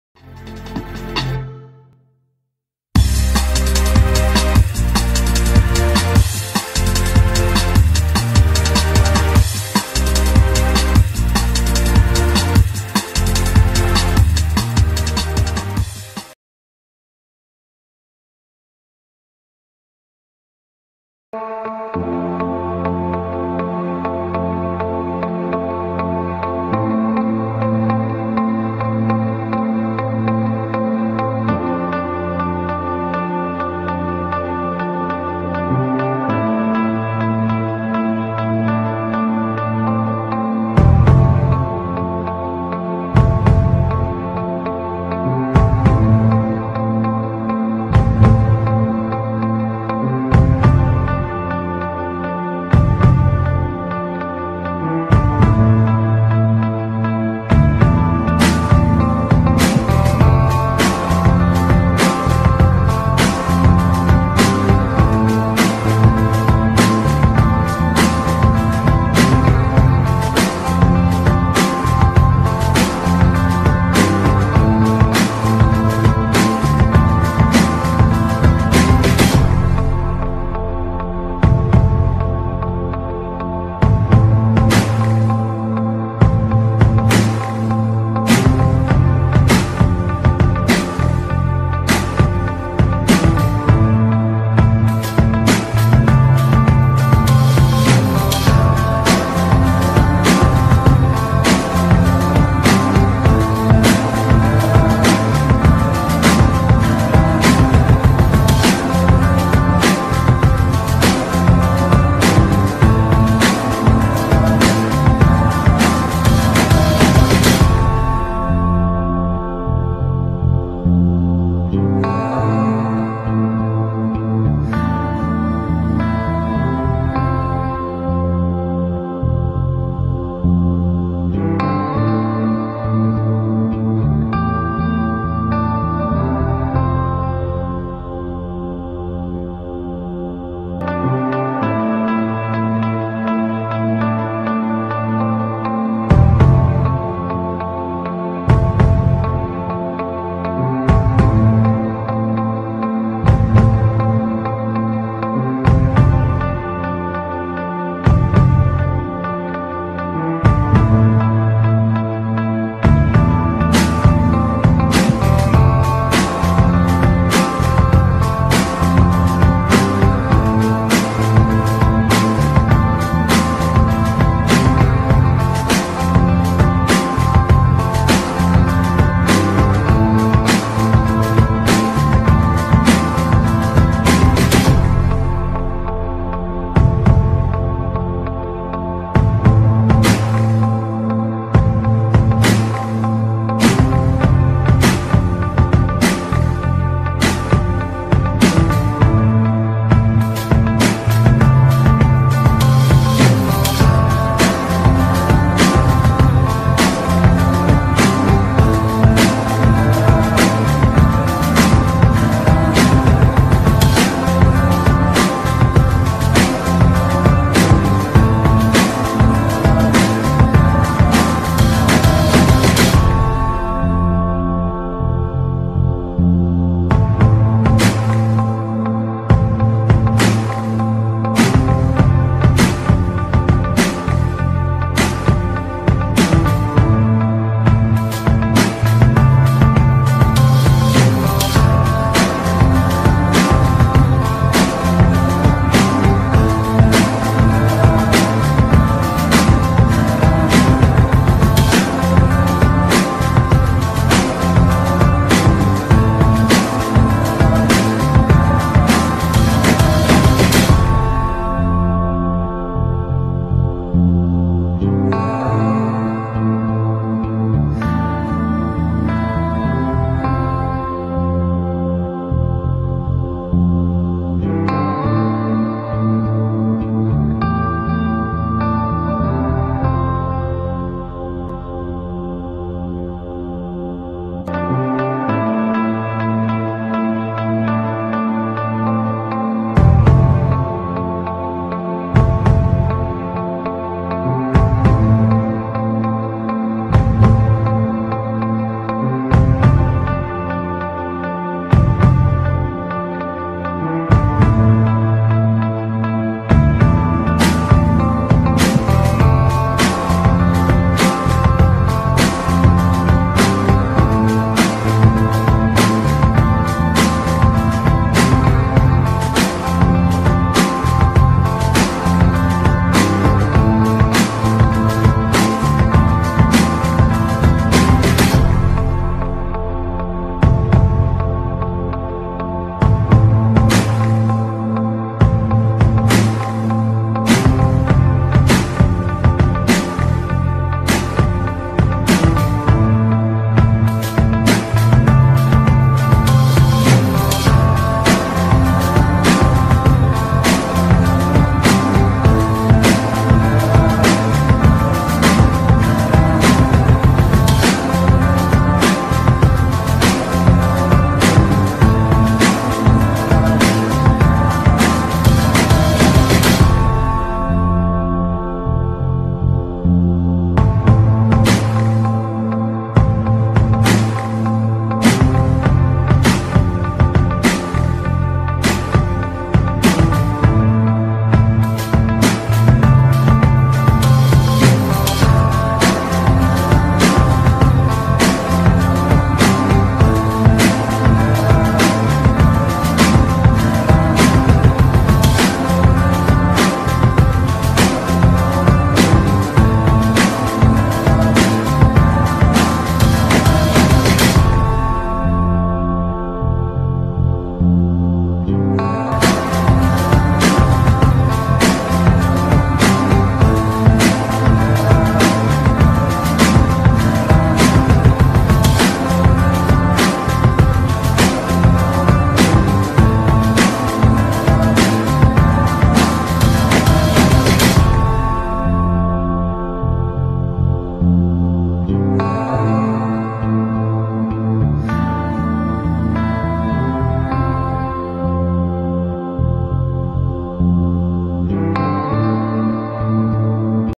Nuno Soares / Nuninho - Highlights 2015/16
Season 2015/16 Best Moments
Nuninho (LW/RW)
33Games
11Assists
7Goals
C.D.Cinfães